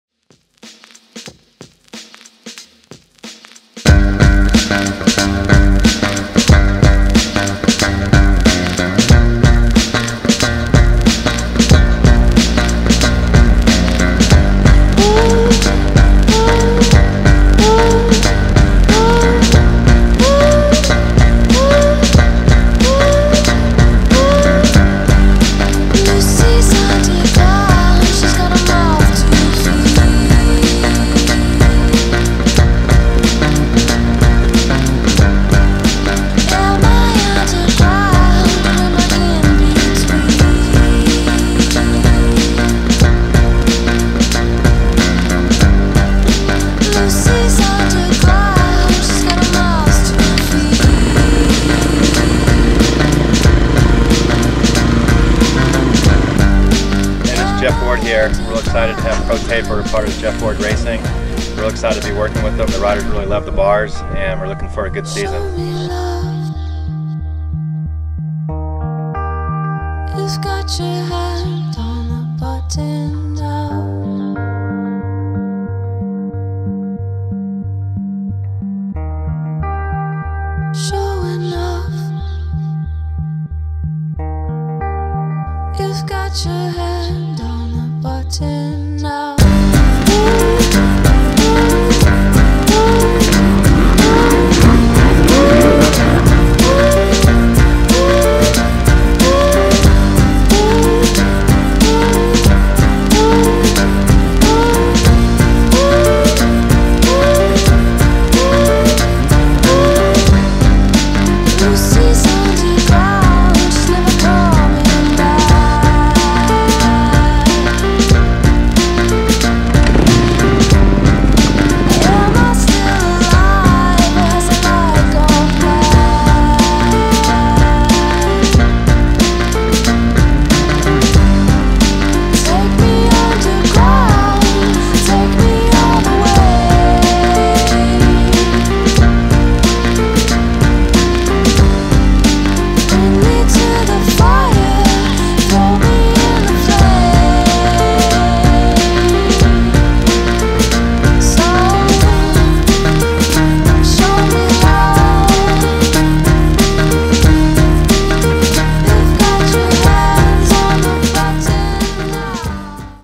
Jeff Ward Racing Intro
Pro Taper introduces the 2012 Jeff Ward Racing Kawasaki Team